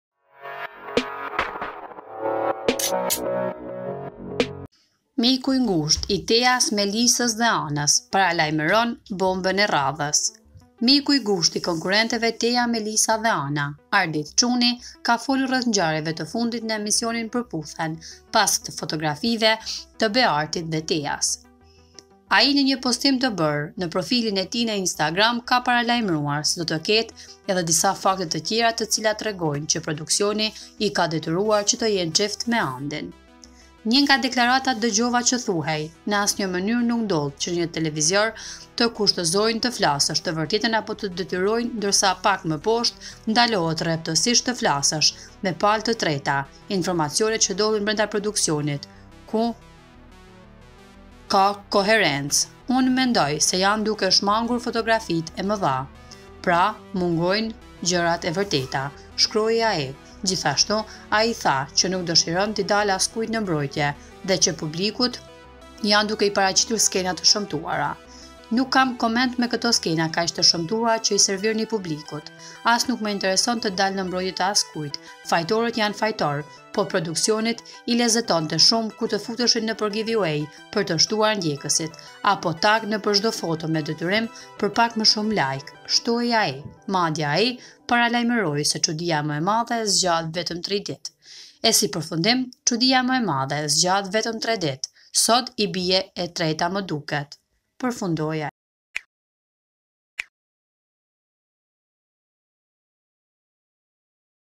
Ardit Cuni Paralajmeron "Bomben" e rradhes
Miku i ngushtë Ardit Cuni i konkurrenteve Tea, Melisa dhe Ana, Ardit Cuni ka folur rreth ngjarjeve të fundit në emisionin “Për’puthen”, pas fotografive të Beartit dhe Teas.

Një nga deklaratat dëgjova që thuhej, ‘Në asnjë mënyre nuk ndodh që në një TV të të kushtëzojnë të flasësh të vërtetën apo të detyrojnë’ ndërsa pak më poshtë ‘Ndalohet rreptësishtë të flasësh me palë të treta informacione që ndodhin brenda produksionit, ku koherenca?, unë mendoje se janë duke munguar fotografitë e mëdha”,